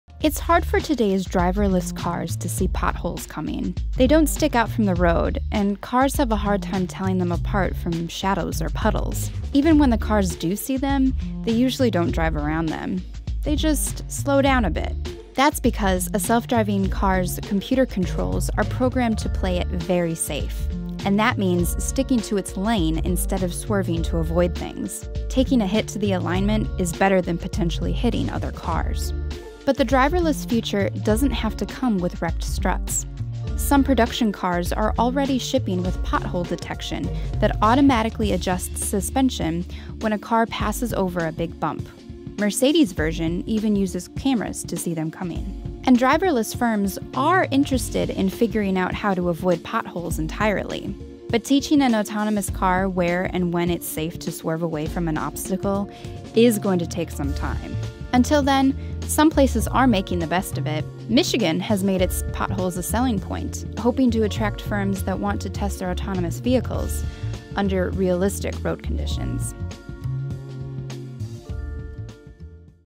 Autonomous cars don't even try to avoid potholes
Today's autonomous cars aren't smart enough to avoid every pothole. They're also programmed to take the hit instead of endangering other drivers.